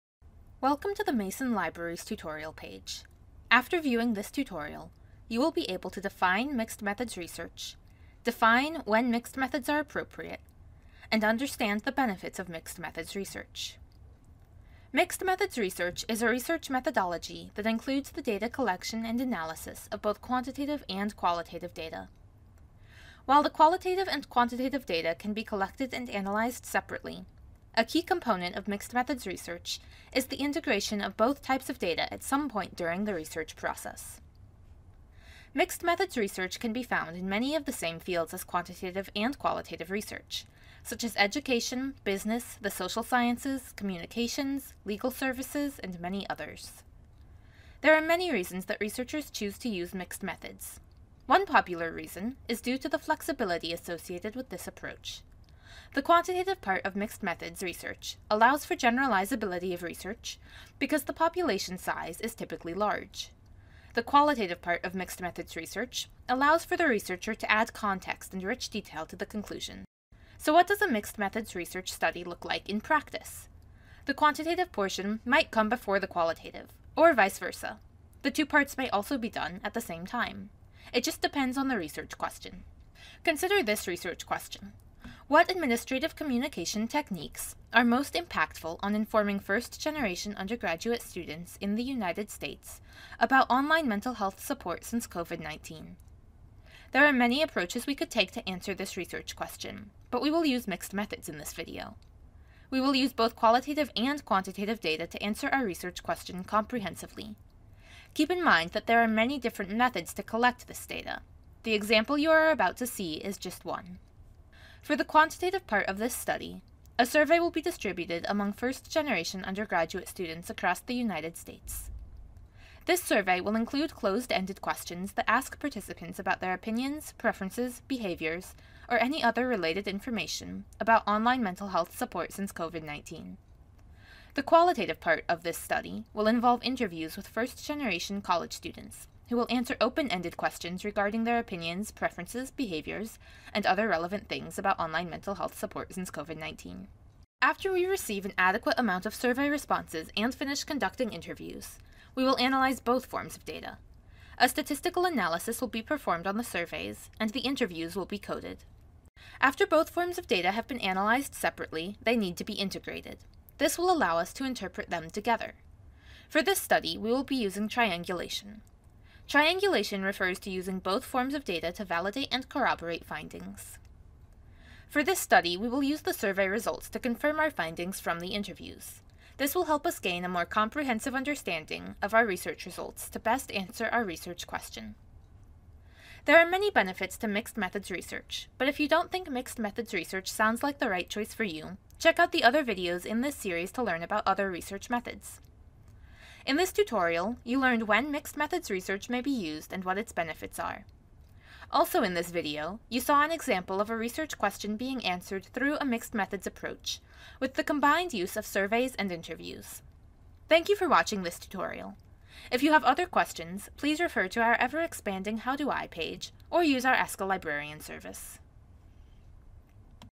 Mixed Methods Research
What is mixed-methods research? When and how do you use it? Watch this video to answer these questions and to see an example of mixed-methods research in action.